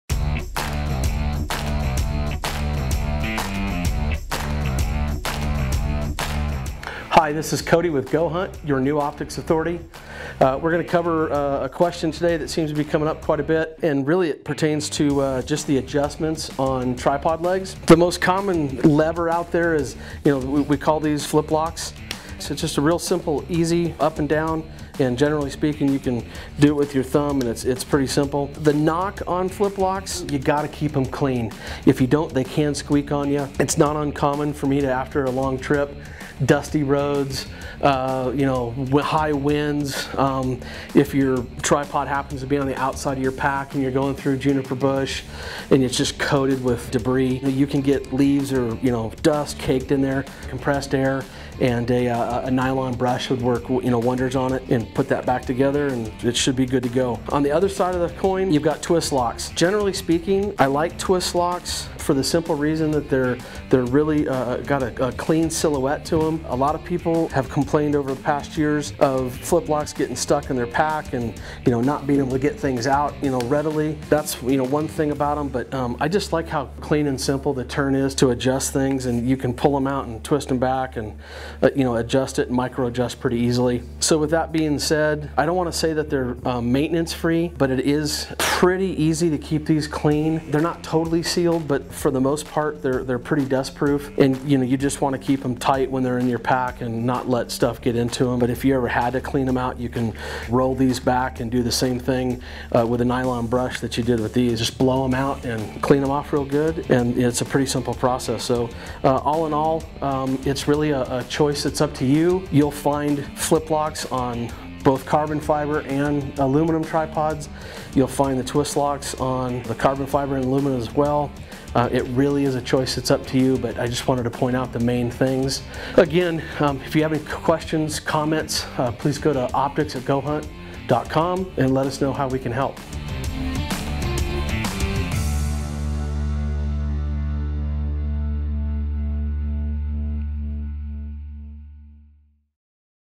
Flip Lock Tripods VS. Twist Lock Tripods
Cody (optics guru) Nelson is back again! In this video, Cody dives into the debate between flip lock and twist lock tripods.